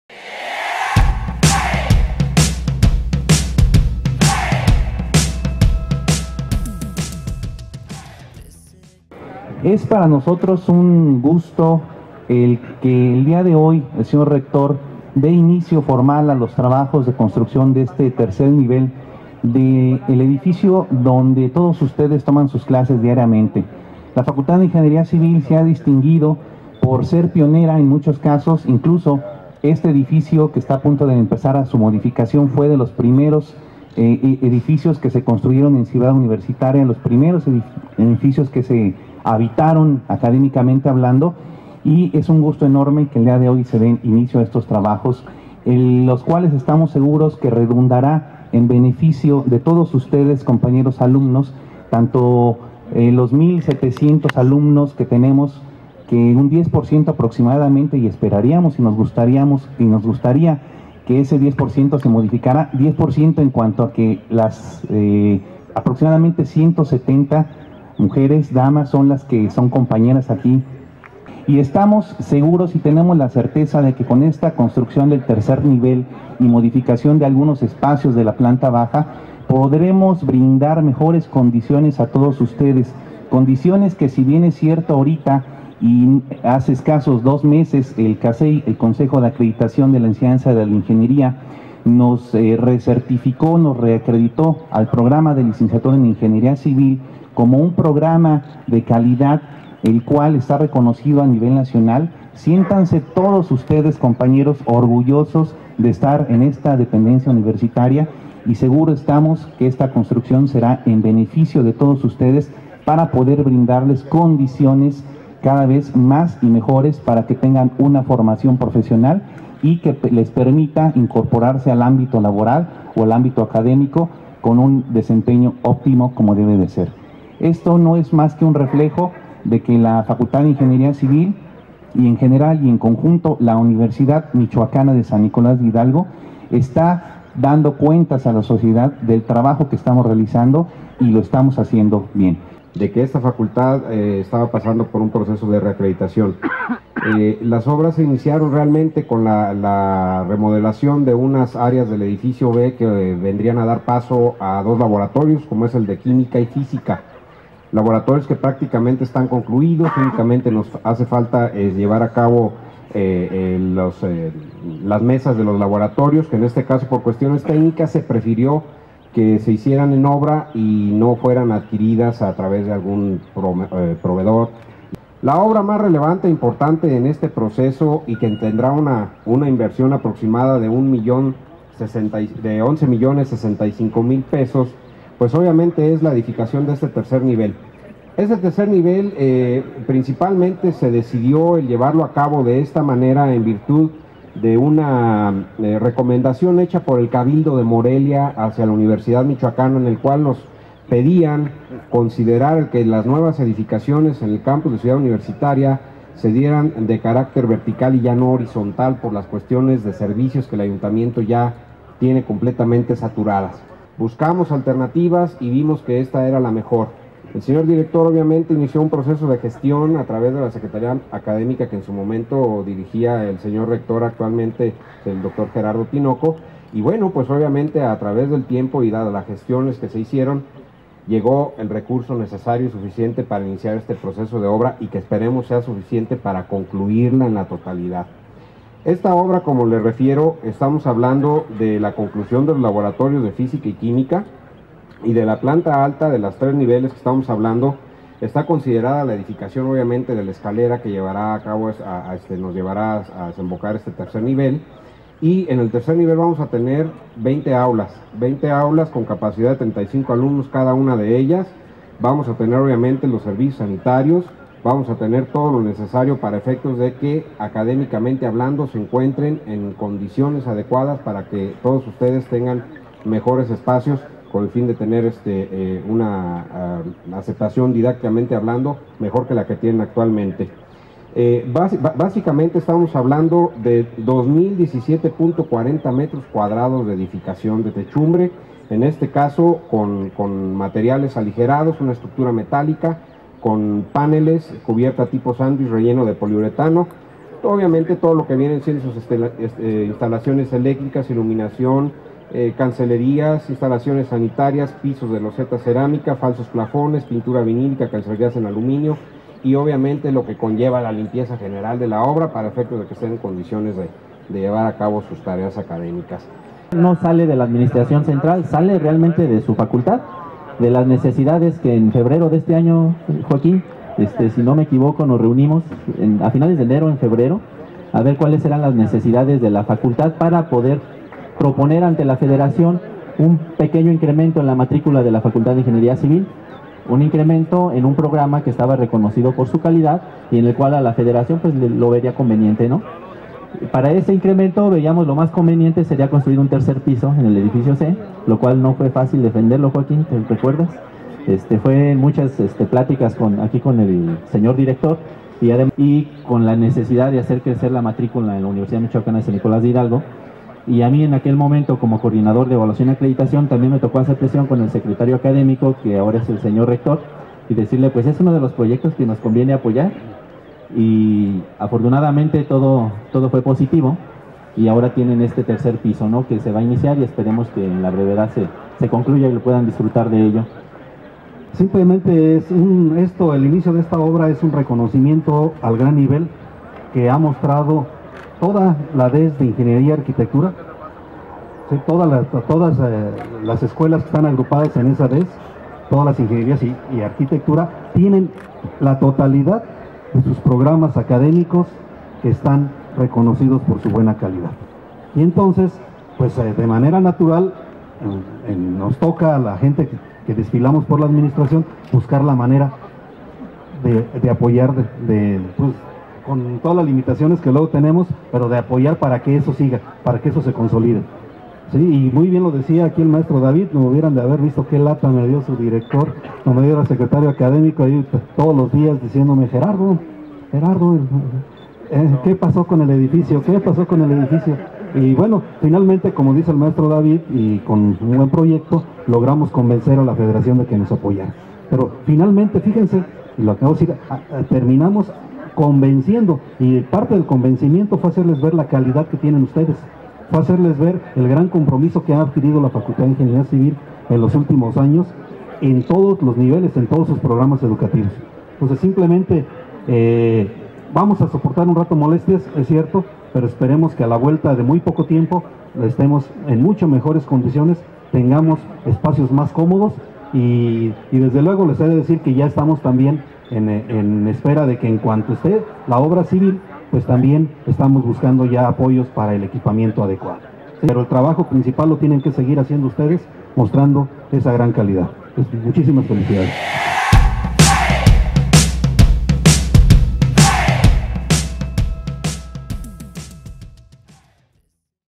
ARRANCAR LA CONSTRUCCIÓN DE Y LA AMPLIACIÓN EN EL EDIFICIO DE LA FACULTAD DE INGENIERÍA CIVIL
Mensajes:
*M.C. JOAQUÍN CONTRERAS LÓPEZ
DIRECTOR FACULTAD DE INGENERIA CIVIL

*ARQUITECTO. LEONEL MUÑOZ BOLAÑOS
COORDINADOR DE PROYECTO Y OBRAS

*MTRO. DAVID X. RUEDA LÓPEZ
SECRETARIO ACADÉMICO UMSNH

*DR. JOSÉ GERARDO TINOCO RUIZ
RECTOR DE UMSNH